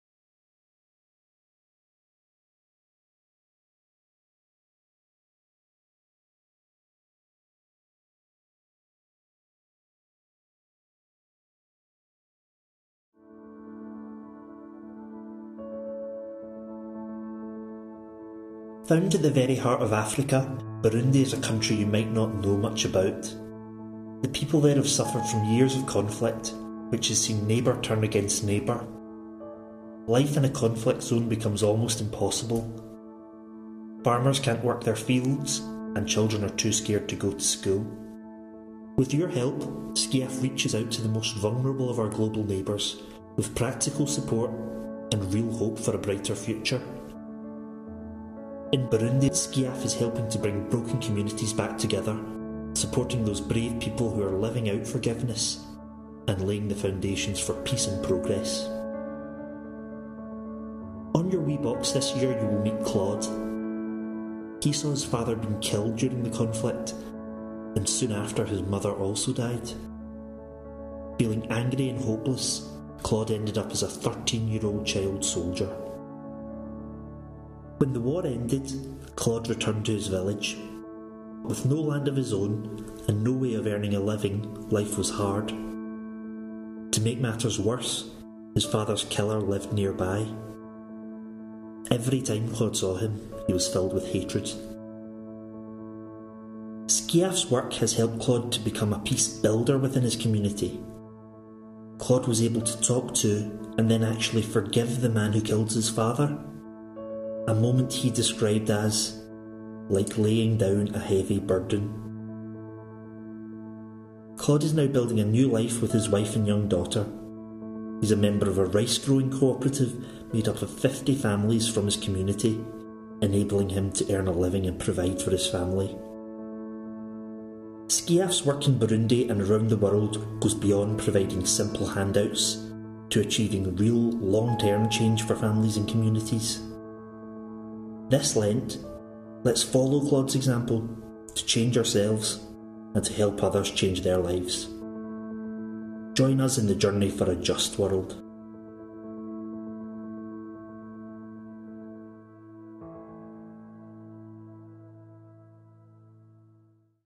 WEE BOX 2013 - Claude's Story
Discover the story of former child soldier Claude, who has transformed his life following a difficult start in Burundi